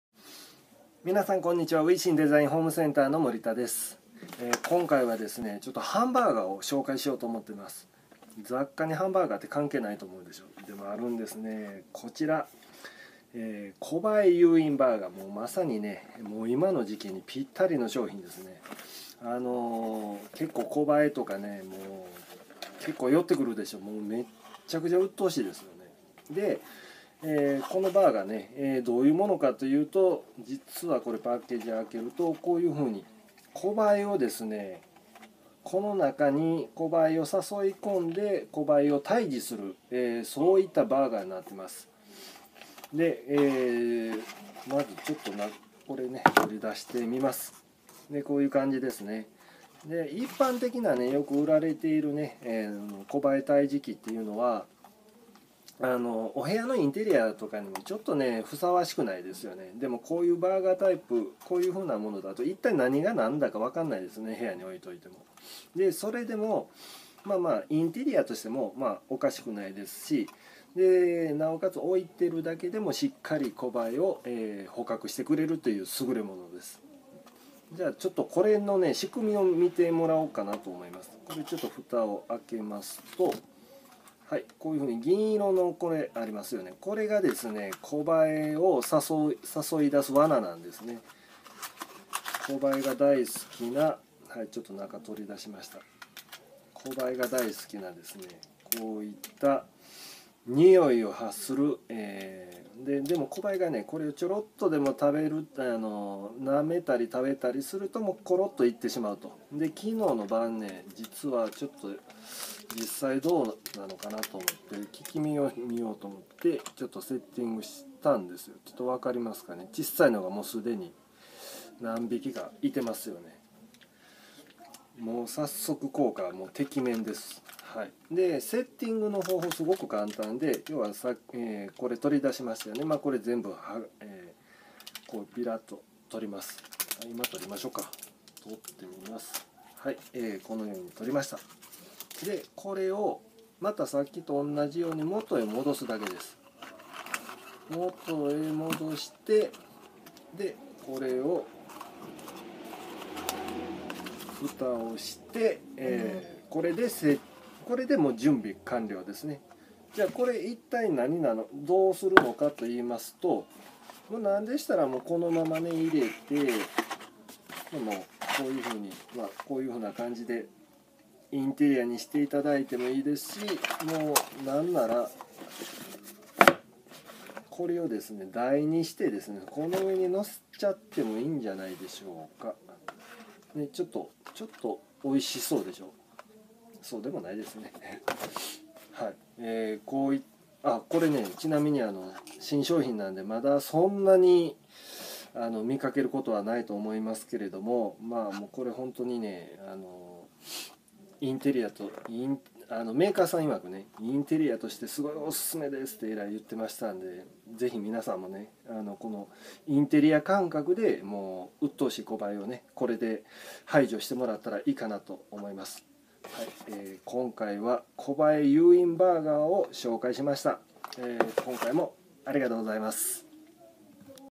コバエユーインバーガー 大阪大人デザイン雑貨店UISIN
ハンバーガーがコバエを食べる！？インテリアとして使えるコバエ捕獲バーガーです。うっとしいコバエもこれを部屋に置いておくだけでいなくなり、インテリアのオブジェとして使えます。効能は1か月で、そのまま手を汚さず普通ごみで捨てれます。
詳しくは
住所　〒557-0052大阪市西成区潮路1-2-27第2丸美マンション1F店舗